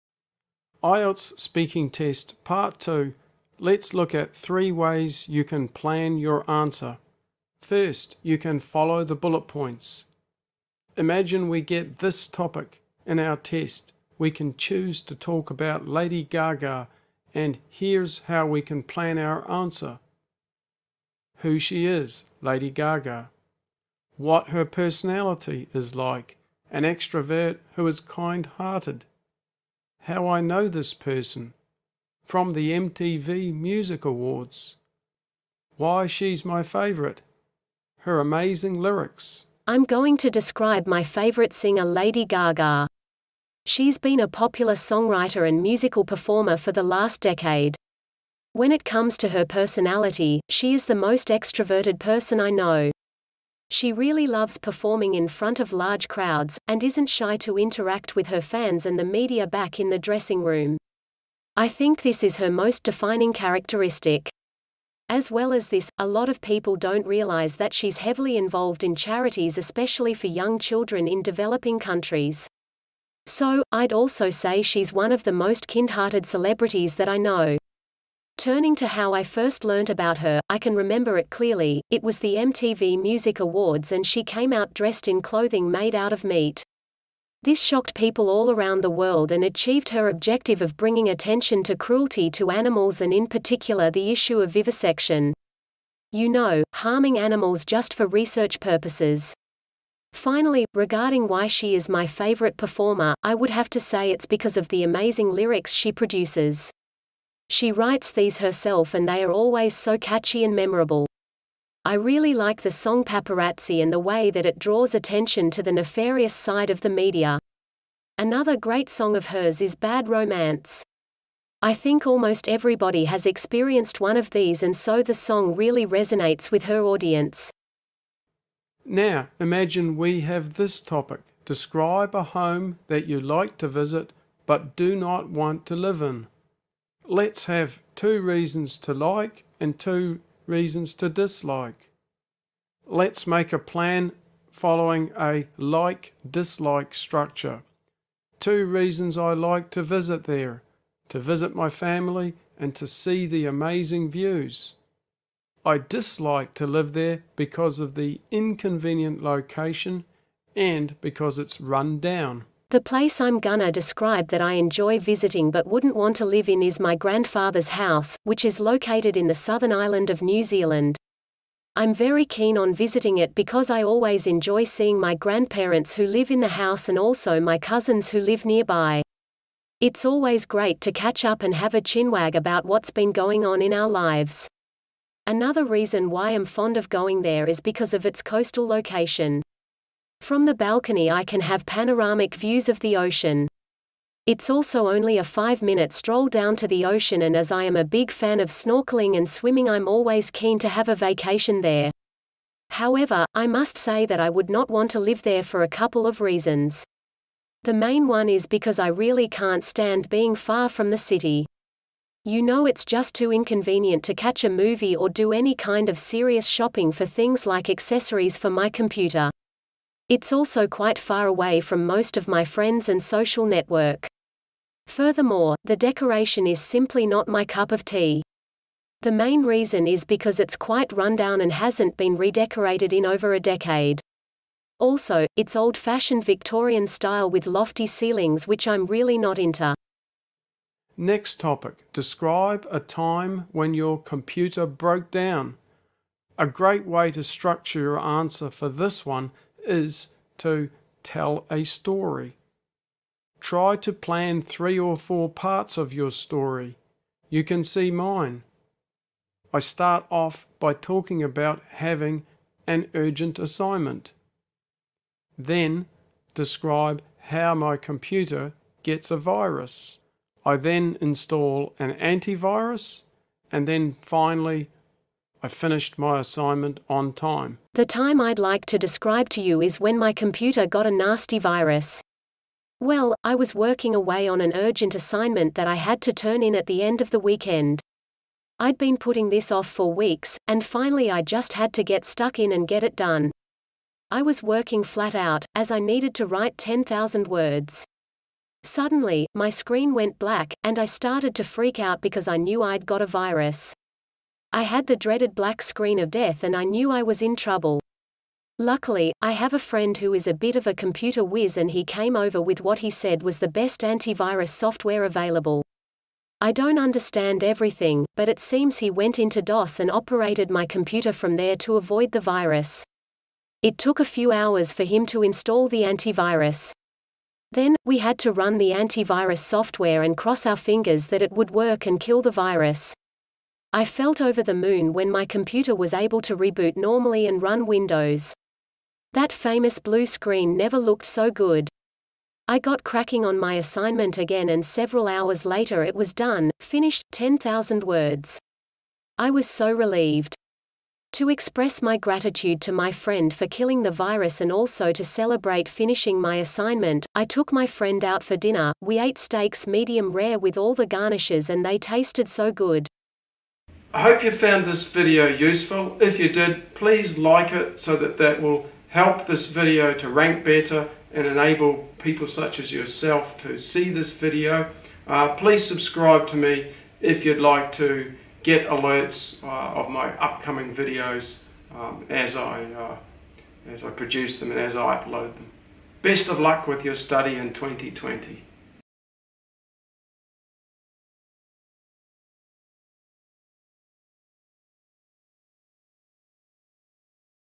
IELTS speaking part 2 PLANNING methods
Three methods for planning your part 2 speaking answer are given. A model answer using each plan is given.  Prepare your answer so that you can speak fluently for two minutes.

METHODS
1. follow the bullet points
2. likes and dislikes
3. tell a story